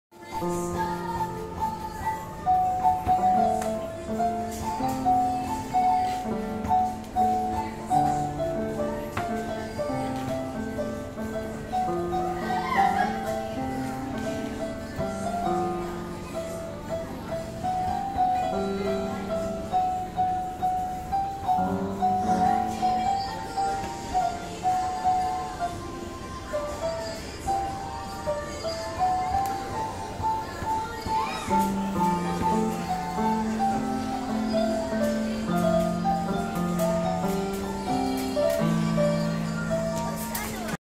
Fun play on Giant floor Piano in London toy shop_2020
fun attempt on Floor piano